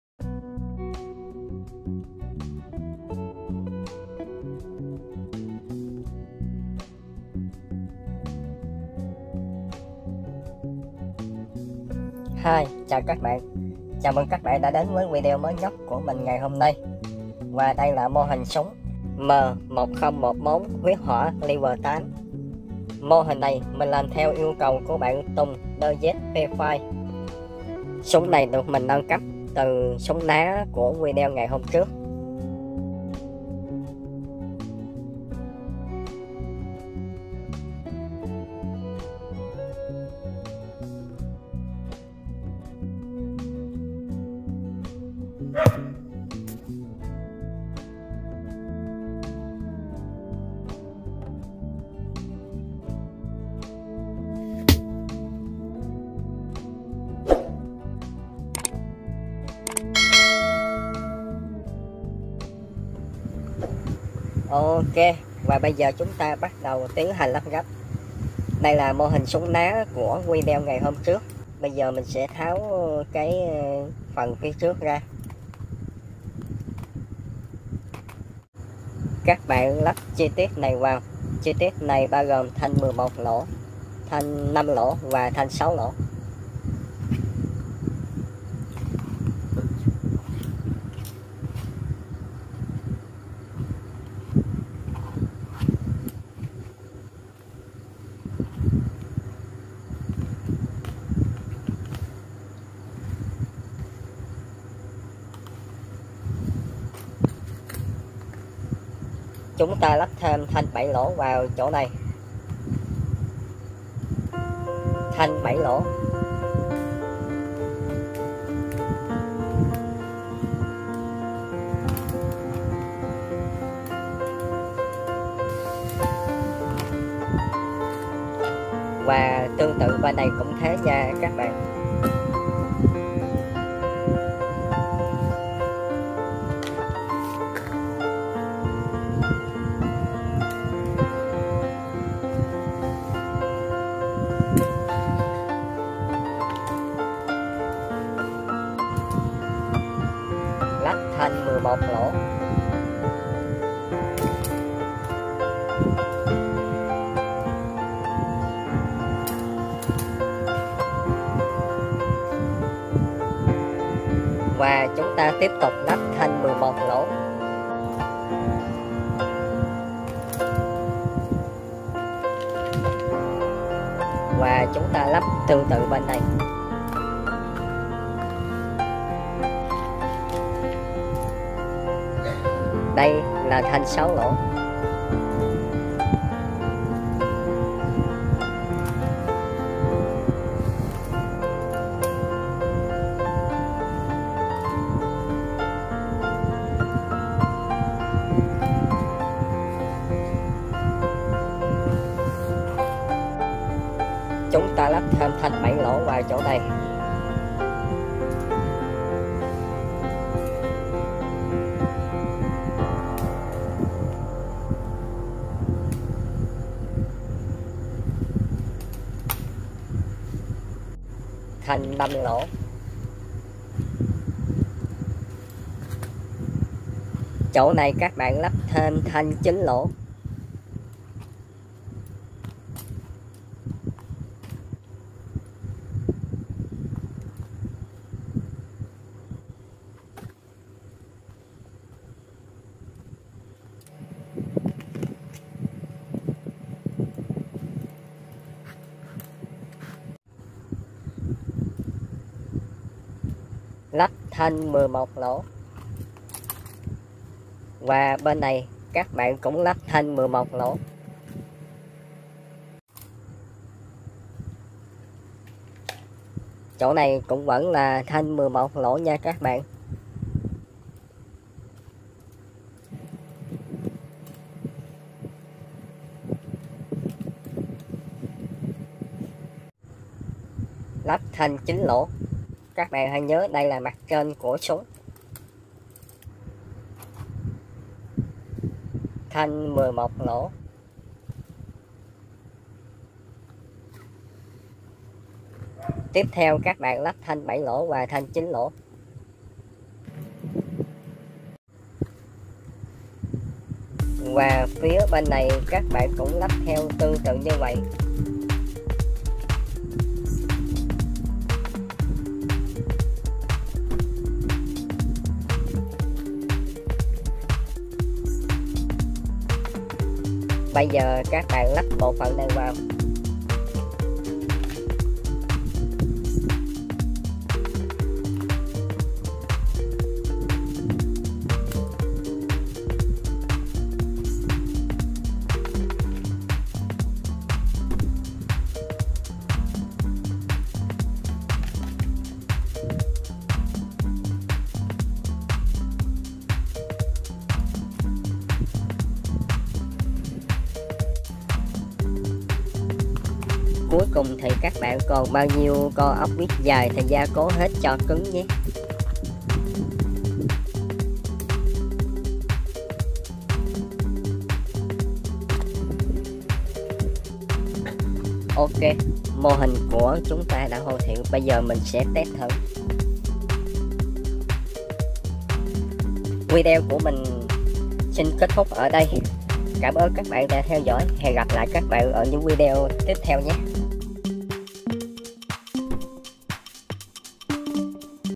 Cải tiến ná cao su thành M1014 huyết hoả lv 8, từ bộ lắp ghép.
Chào các bạn, hôm nay mình sẽ cải tiếng ná cao su của video trươc thành s.úng M1014 huyết hoả lv8 từ bộ lắp ghép, đây là một mô hình tương đối đơn giản và dễ làm, chúc các bạn và các em xem video vui vẻ và làm theo thành công, một trò chơi cực kỳ giải trí trong dịp hè này